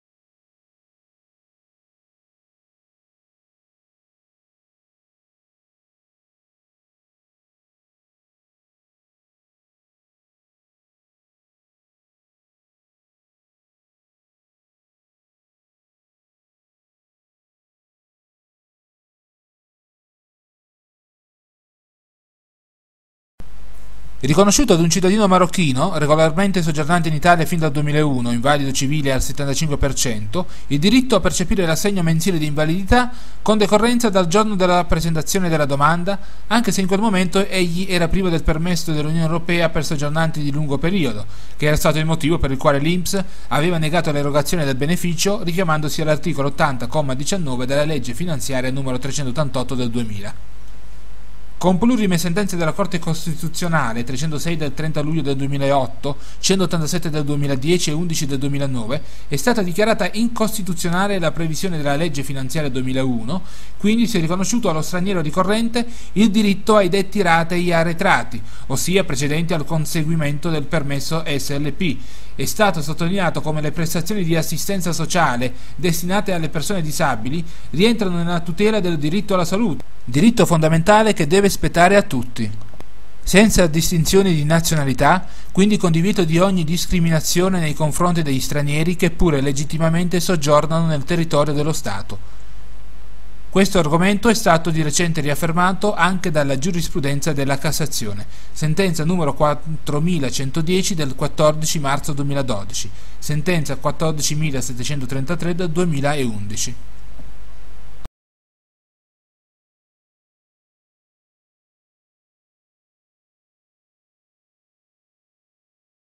Straniero disabile diritto assegno invalidità da data domanda, non da rilascio pds SLP
Tribunale di Perugia, Sezione Lavoro, Sentenza del 15 marzo 2012

Il Tribunale di Perugia ha riconosciuto  ad un cittadino marocchino, regolarmente soggiornante in Italia dal 2001, invalido civile nella misura del 75%, il diritto a percepire  l'assegno mensile di invalidità, con decorrenza dal giorno della presentazione dell'istanza amministrativa anche se in quel momento egli era privo del  permesso UE per soggiornanti di lungo periodo, motivo per il quale l'INPS aveva negato l'erogazione del beneficio, richiamandosi all'art. 80 c. 19 della legge n. 388/2000.
Successivamente al deposito del ricorso, il ricorrente aveva conseguito il permesso UE per lungo soggiornanti e l'INPS aveva erogato il beneficio richiesto, ma con decorrenza dalla data del  rilascio del permesso UE e non della domanda iniziale.

Il Giudice ha fatto riferimento alla nota giurisprudenza della  Corte Costituzionale che dalla sentenza n. 306 dd. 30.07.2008 fino alle sent. n. 187/2010  e n. 11/2009, ha dichiarato l'illegittimità costituzionale della norma citata della legge finanziaria 2001 e ha pertanto riconosciuto allo straniero ricorrente il diritto ai detti ratei arretrati. Nelle citate sentenze, il giudice delle leggi  ha infatti sottolineato come  le prestazioni di assistenza sociale destinate alle persone disabili rientrano nella tutela del diritto alla salute, inteso come diritto ai rimedi possibili alle menomazioni prodotte dalla disabilità; diritto alla salute che, in quanto diritto fondamentale, deve spettare a tutti, senza distinzioni fondate sulla nazionalità, con questo vietandosi ogni forma di discriminazione nei confronti degli stranieri legittimamente soggiornanti nel territorio dello Stato.

Tali argomenti sono stati di recente riaffermati anche dalla  giurisprudenza di Cassazione (Corte di Cassazione, VI sez. civ., sentenza n. 4110 dd. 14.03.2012; sentenza n. 14733/2011, depositata il 5 luglio 2011).